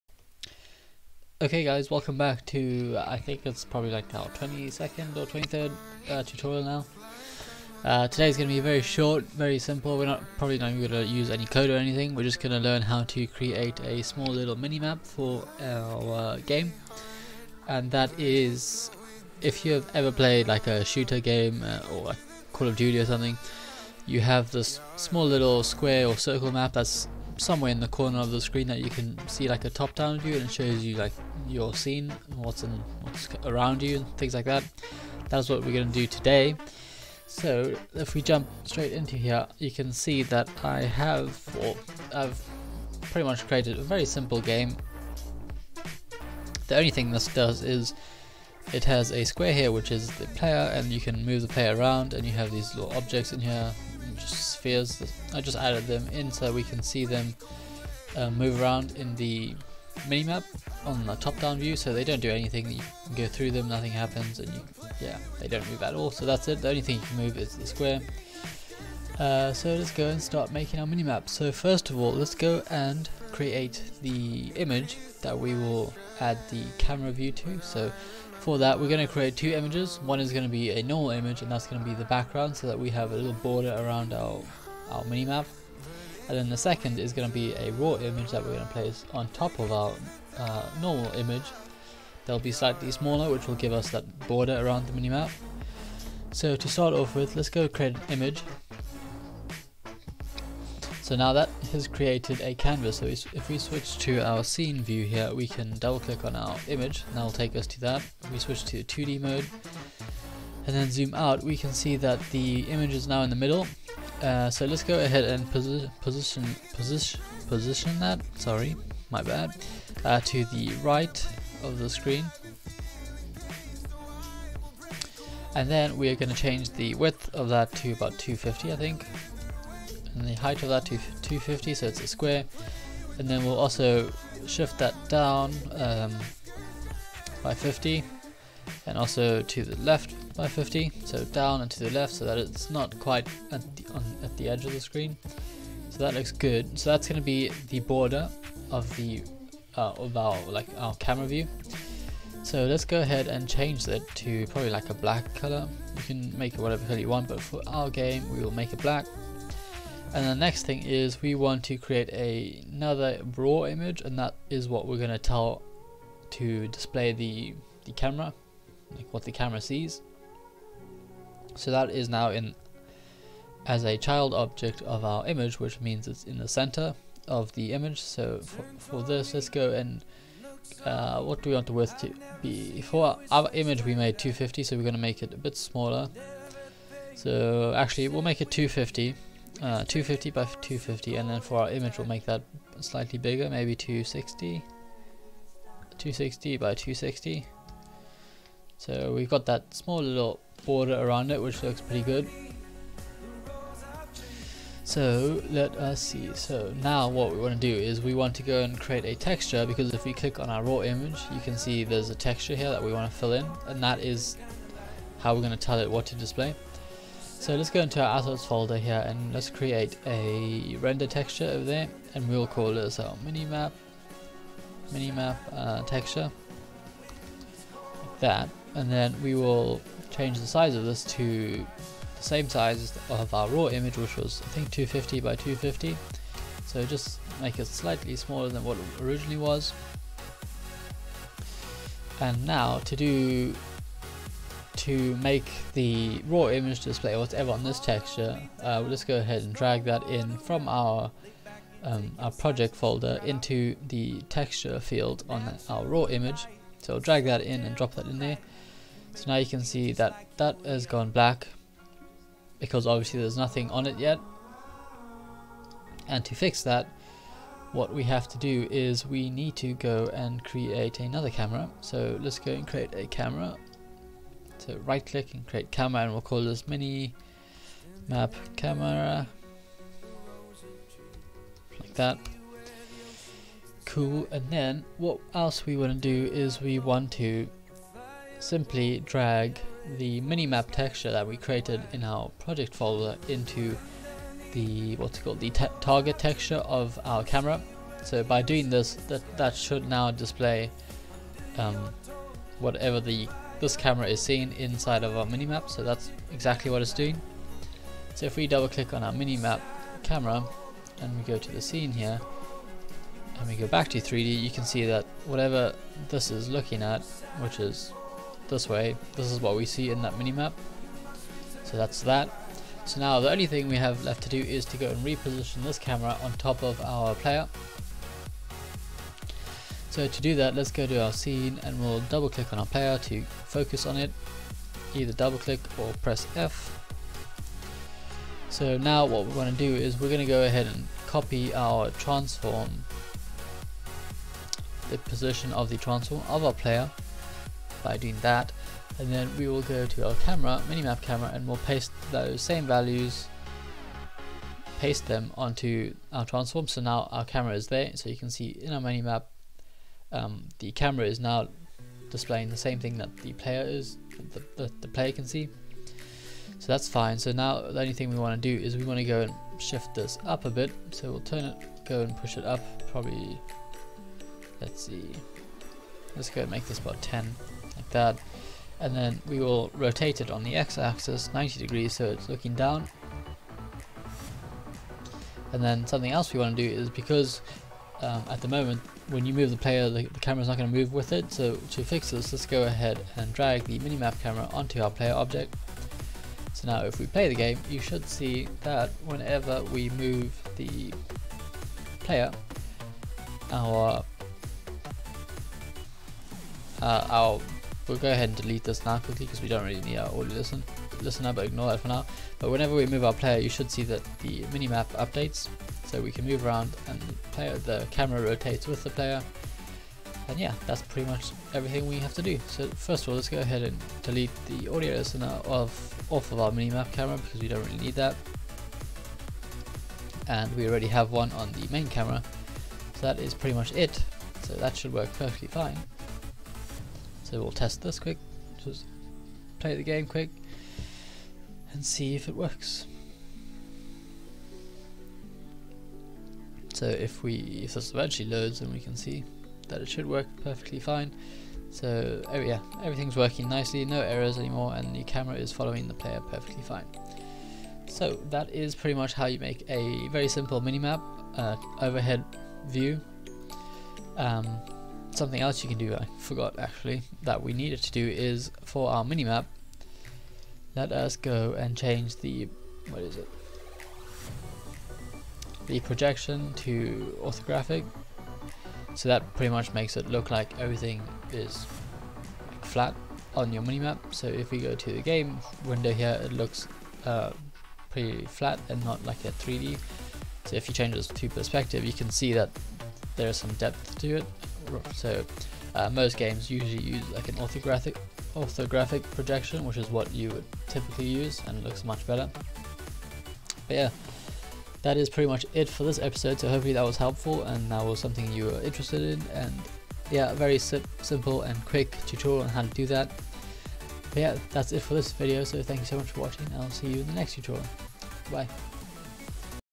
How to create a top down mini map for your unity game
Learn how to create a simple and quick mini map to show a top down view of your game.